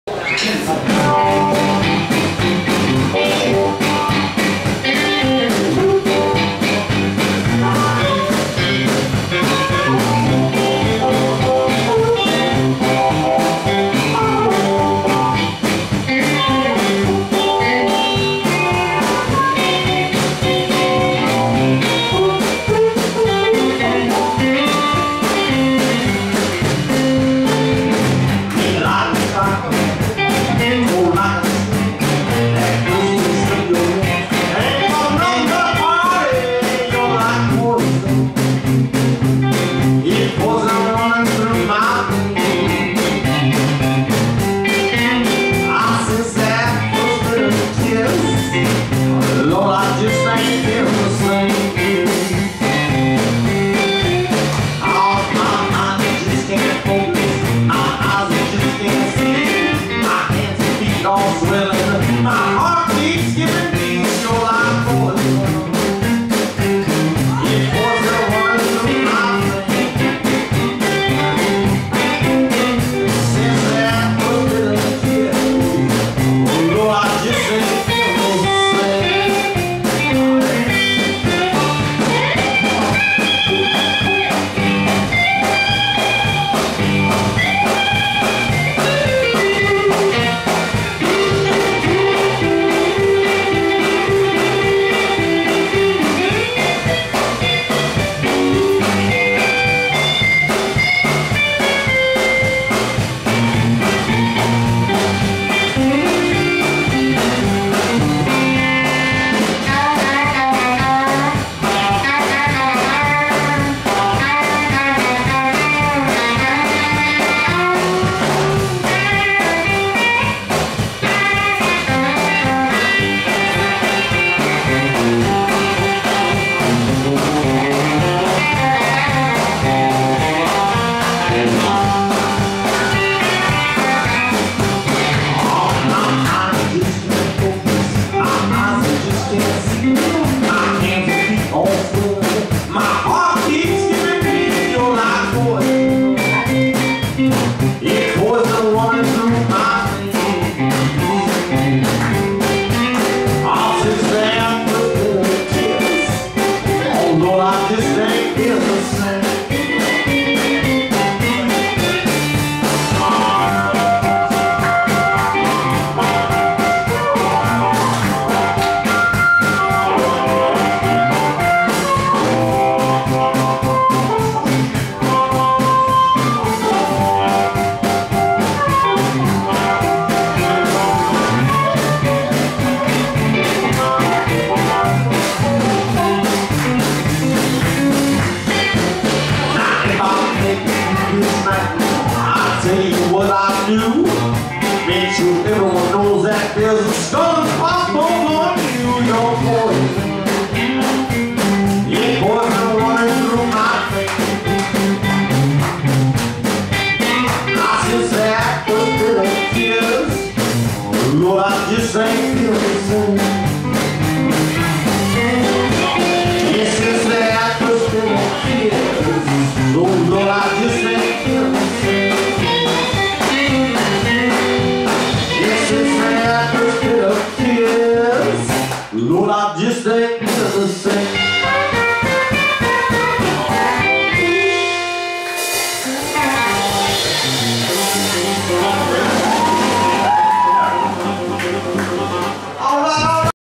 New Song #1 by Morry Sochat & The Special 20s
The Smoke Daddy, Wicker Park, Chicago - April 27, 2012
Morry Sochat on vocals/harmonica, Joel Paterson on guitar, Mike Scharf on Upright Bass,  Marty Binder on drums